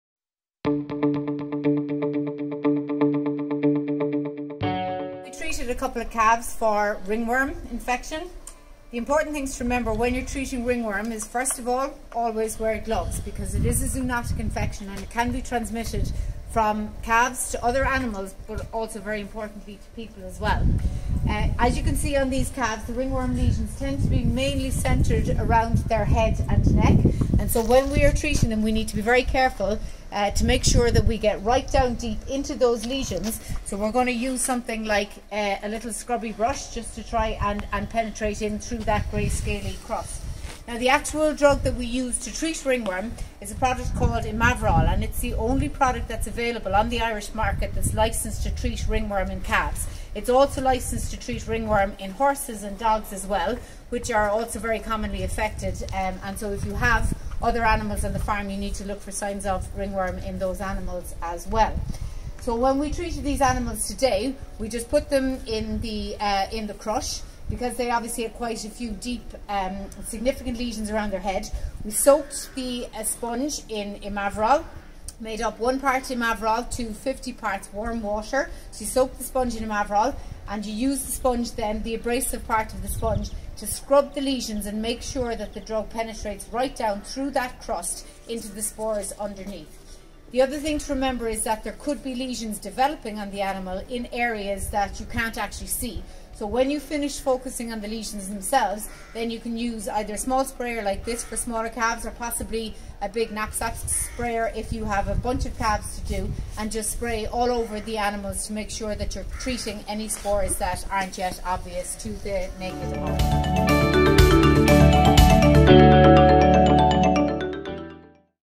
How to treat ringworm with Imaverol
Did you know Imaverol is the only licensed product for the treatment of ringworm in Ireland?
Sinnead Oakes explains how to treat ringworm in calves with Imaverol.
Ringworm is a skin infection caused by various species of microscopic fungi. It can affect cattle, horses, dogs, cats, and other animals. These fungi produce spores, which are microscopic structures that allow the fungi to spread over animal’s coats and throughout its habitat.